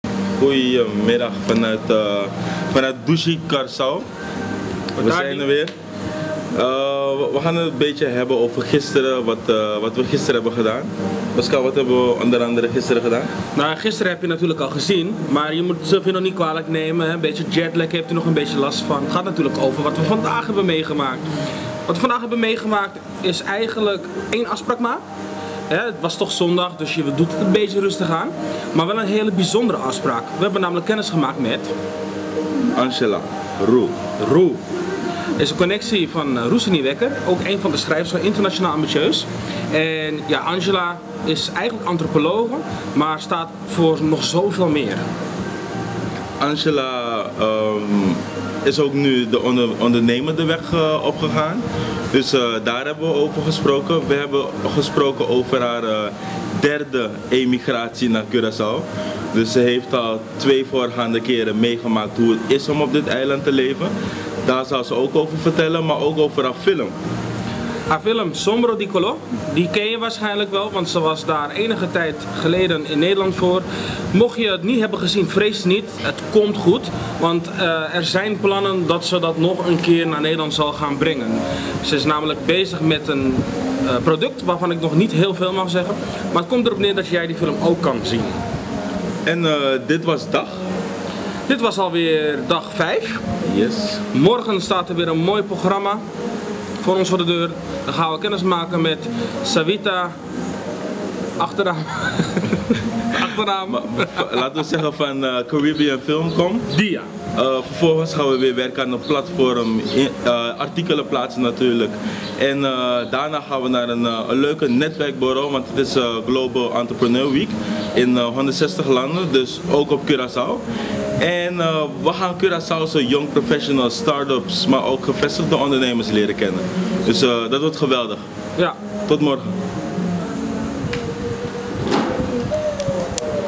IA in Curacao: Dag 5
Vanuit Plaza Hotel (van der Valk) een korte terugblik op de ontmoeting met maker van de documentaire 'Sombra di Kolo' Angela Roe. Mad inspiring!